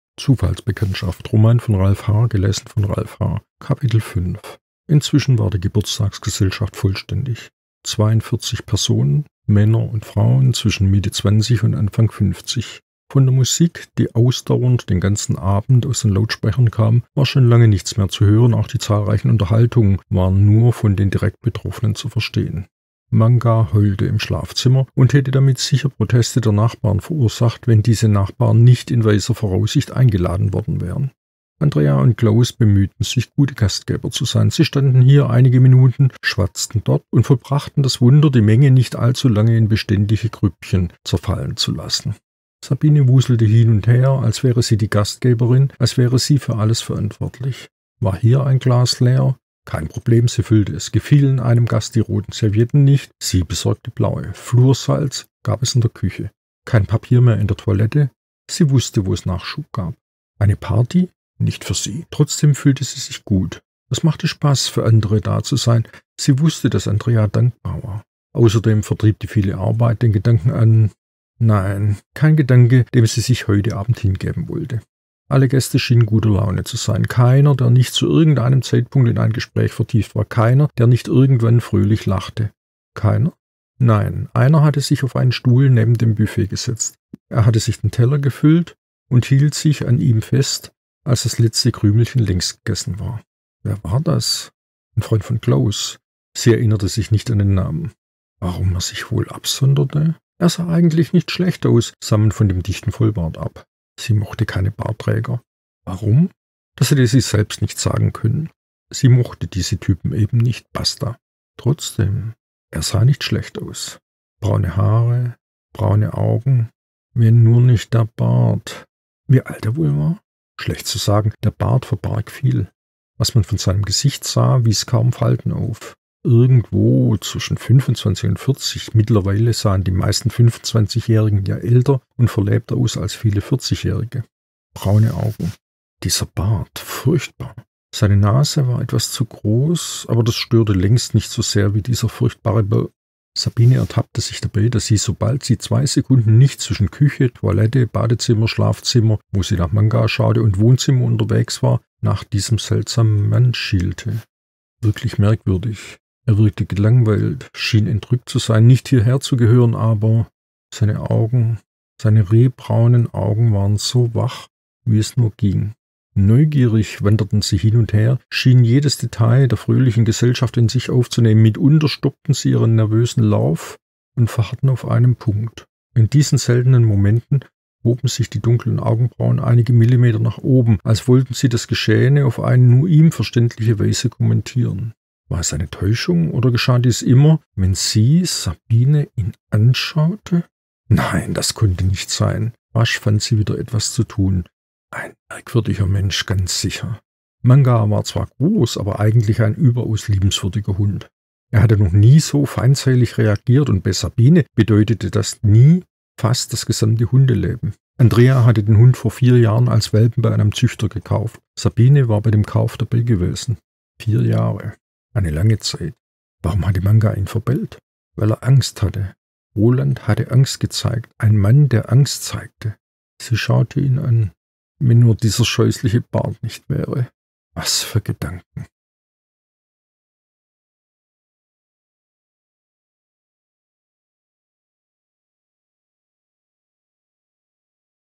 Ralf H. - Zufallsbekanntschaft - Kapitel 5
Kapitel 05 des Romans von Ralf H.

Sabine und Roland begegnen sich auf der Party. Sabine findet den Kerl ziemlich seltsam - und irgendwie auch interessant.

Gelesen vom Autor.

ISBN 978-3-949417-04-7
Überall im Buchhandel erhältlich.